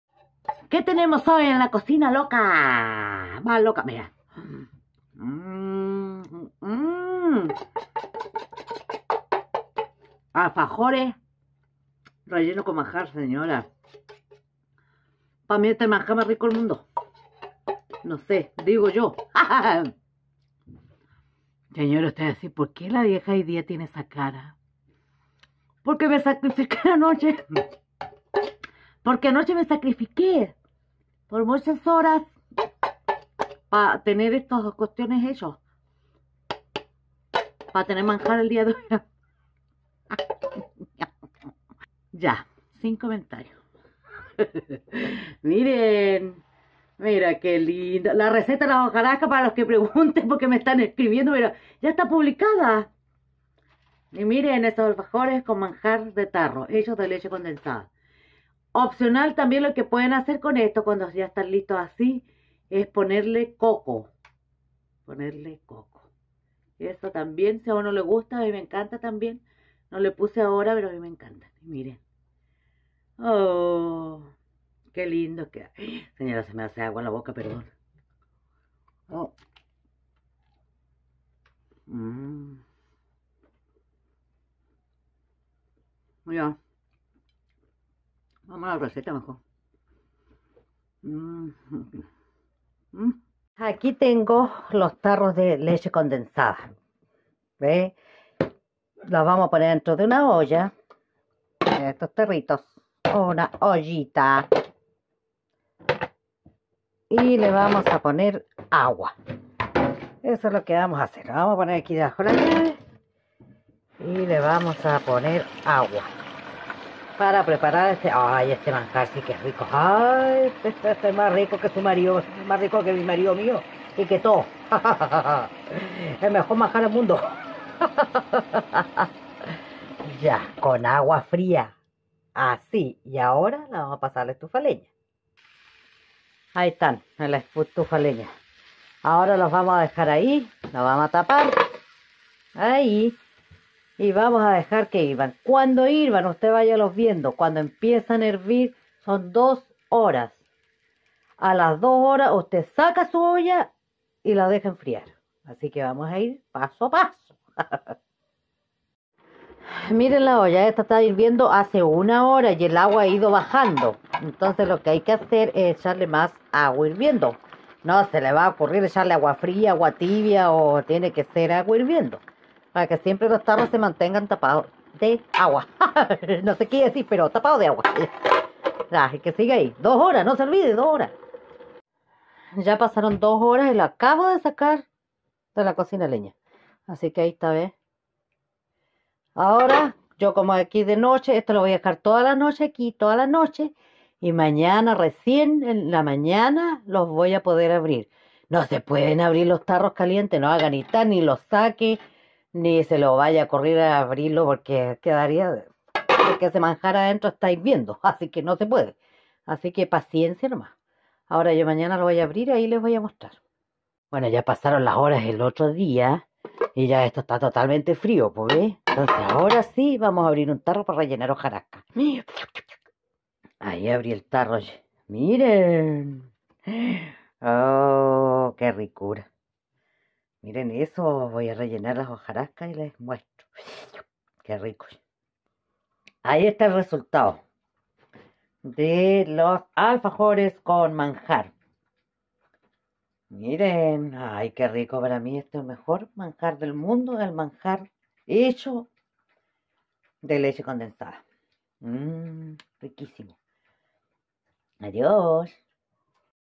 Manjar De Leche Condensada
Para mi este es el mejor manjar del mundo ya que me recuerda mi niñez